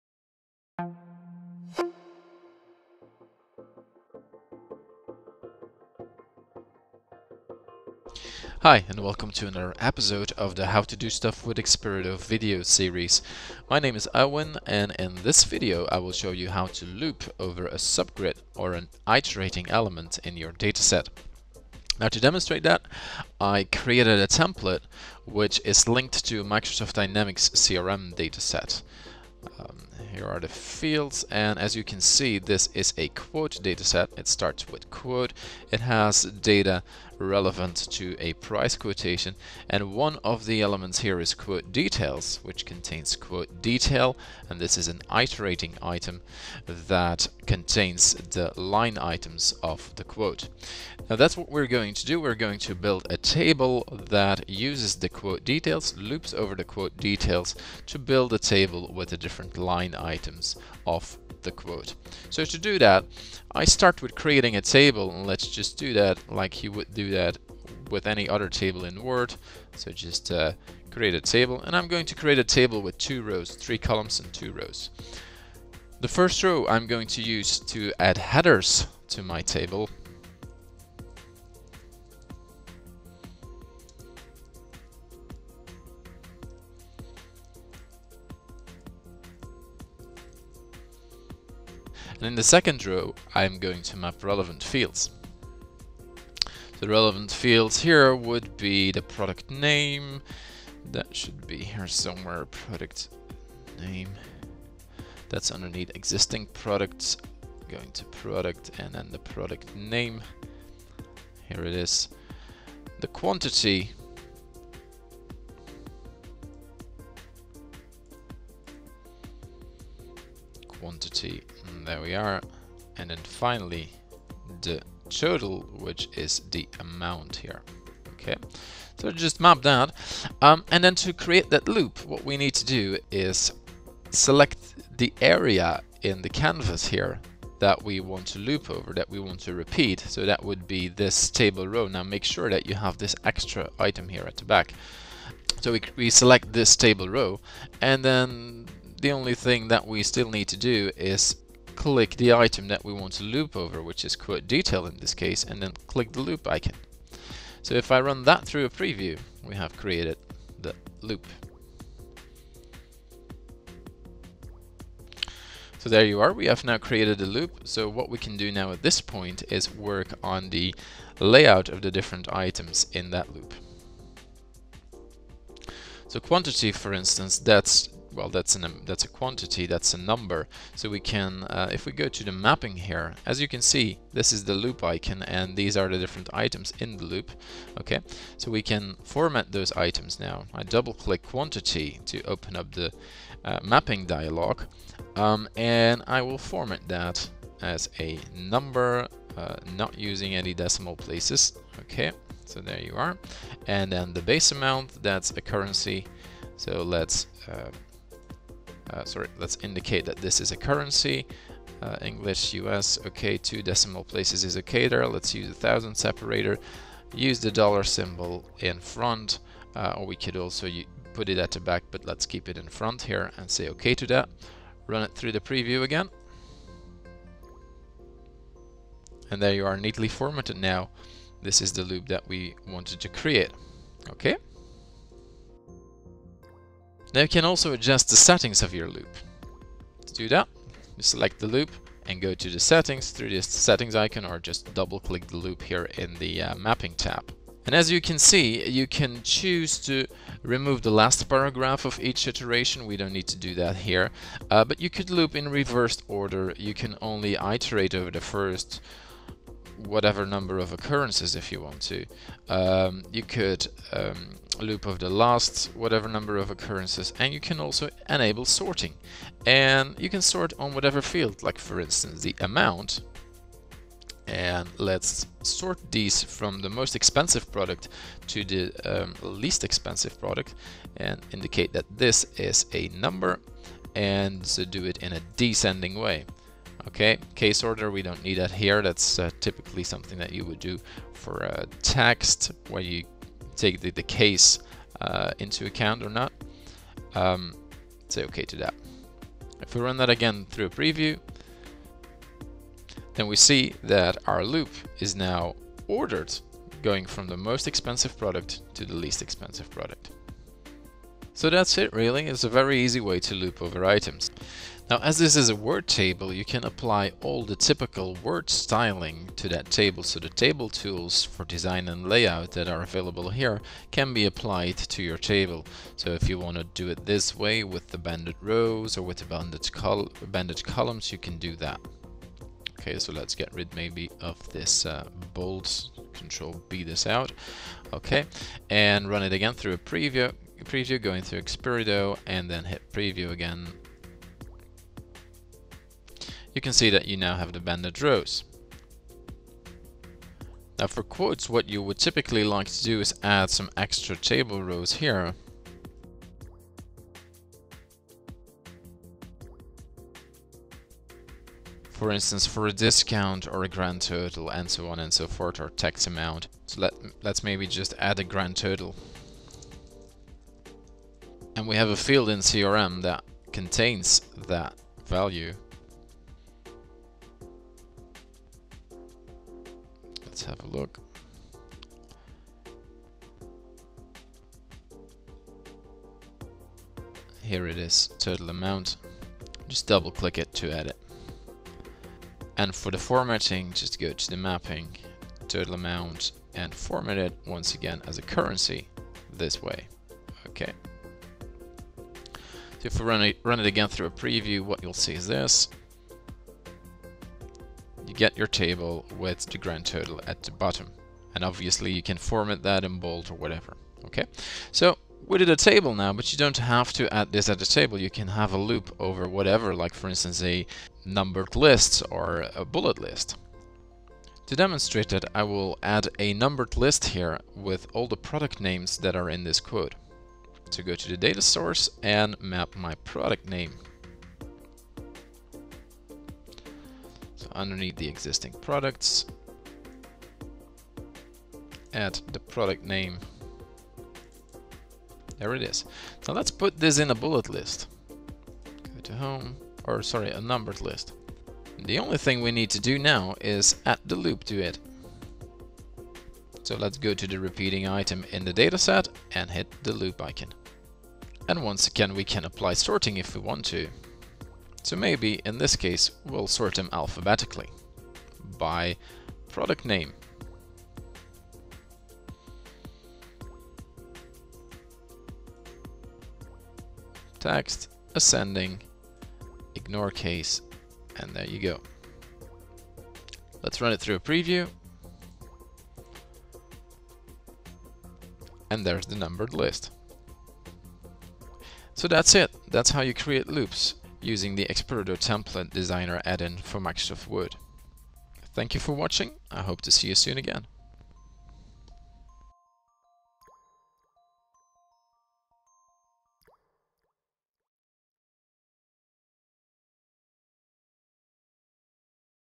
How to do stuff with XperiDo - Looping over an iterating element
Episode of the "How to do stuff with XperiDo" video series, on how to use the XperiDo template design add-in for Microsoft Word to loop over an iterating element.